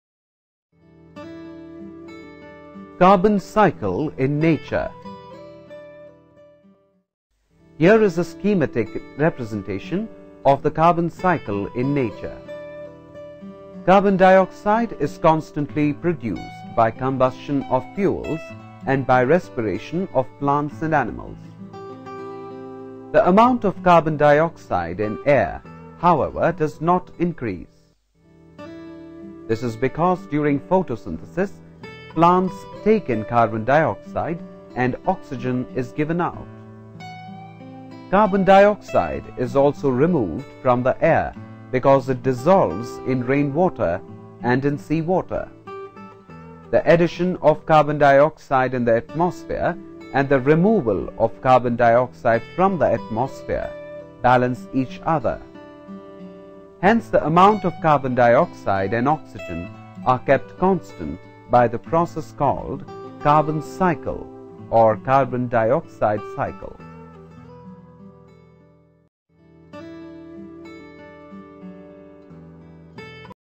Carbon Cycle in Nature
Carbon cycle is one of the important cycle by which carbon present in the different places of earth are exchanged.Carbon is very essential for the life. Carbon is present in the different sphere of Earth

Atmosphere
Hydrosphere
Biosphere